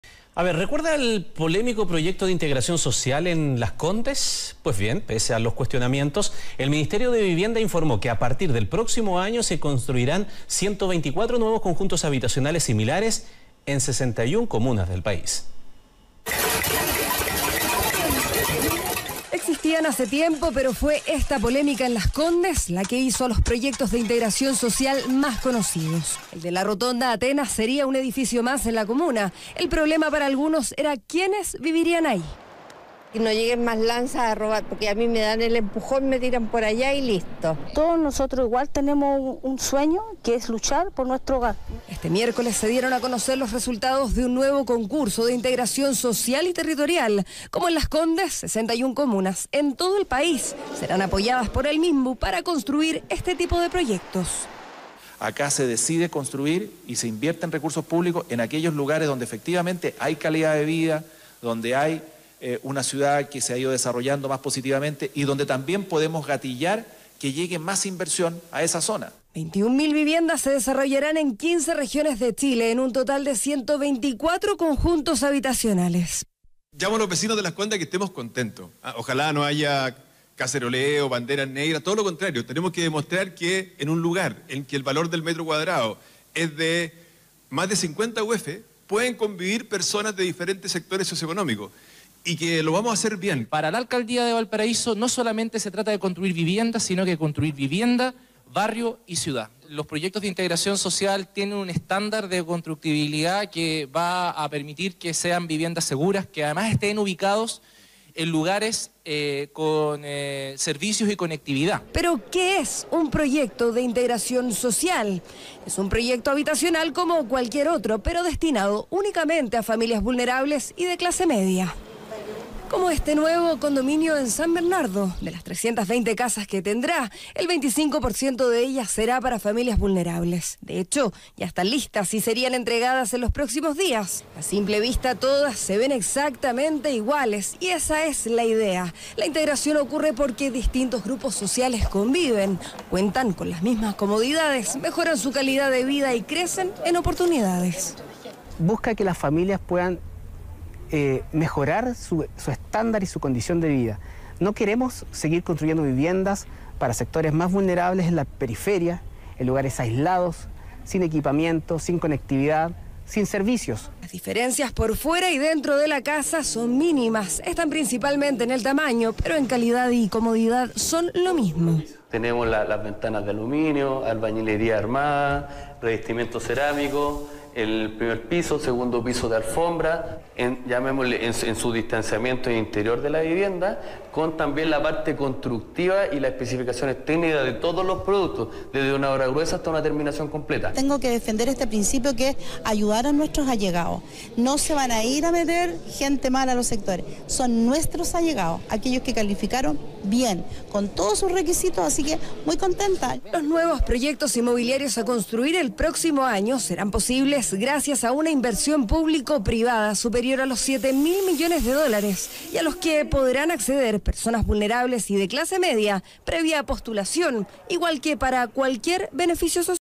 Integración social: 124 nuevos proyectos
¿Recuerda el polémico proyecto de integración social en Las Condes? Pues bien, pese a los cuestionamientos, el Ministerio de Vivienda informó que a partir del próximo año se construirán 124 nuevos conjuntos habitacionales similares en 61 comunas del país.